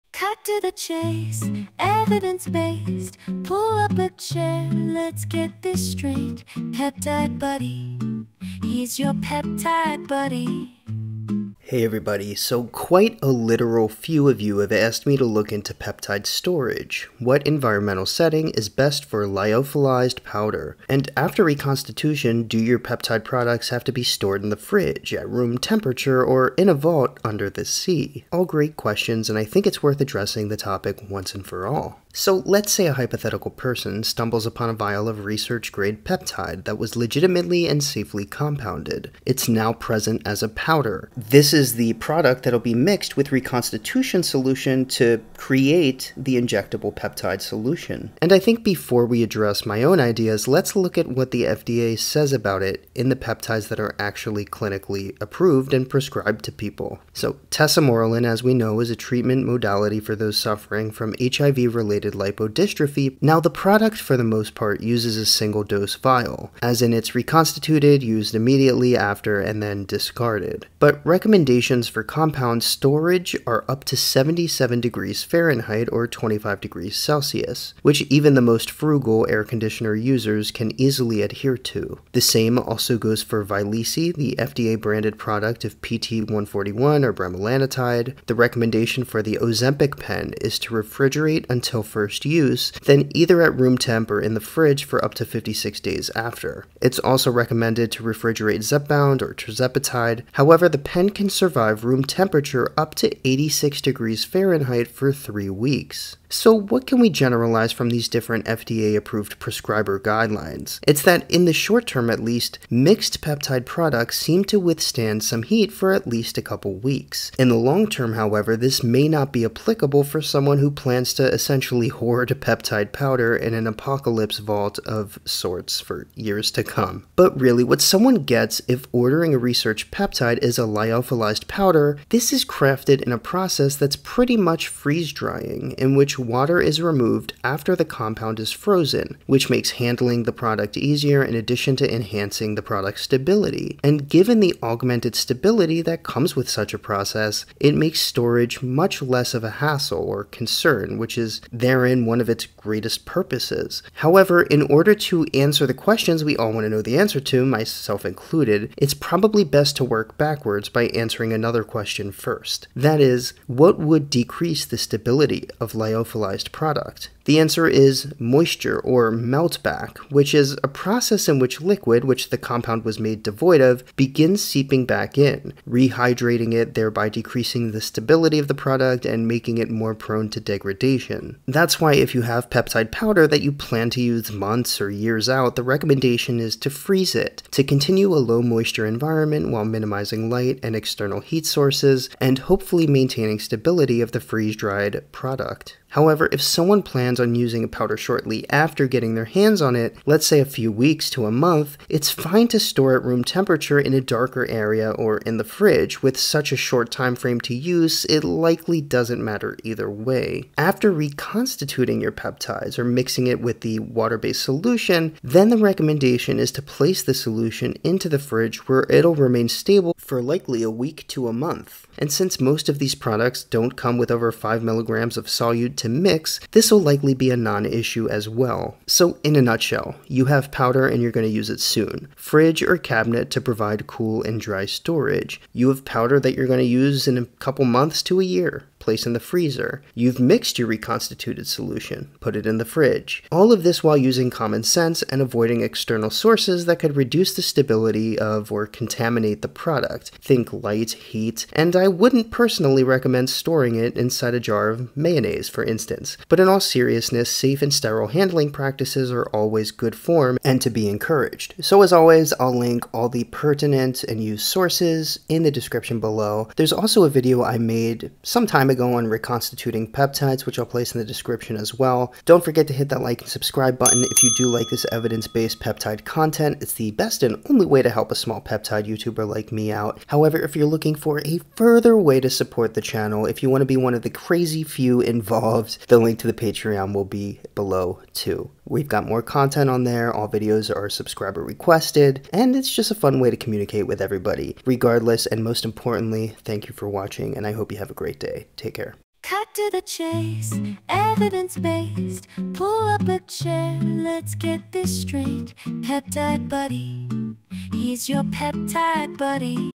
How To Store Your Peptides (For Maximal Use)
Today let's review the heavily requested topic about how to store your peptides.

References:
1. **Principles for Drug Reconstitution**

2. **FDA Label for 210557**

3. **FDA Label for 022505**

5. **How to Use Zepbound**

6. **FDA Guide on Lyophilization**

7. **Optimizing Lyophilization Cycle Times**

8. **Handling and Storage of Peptides**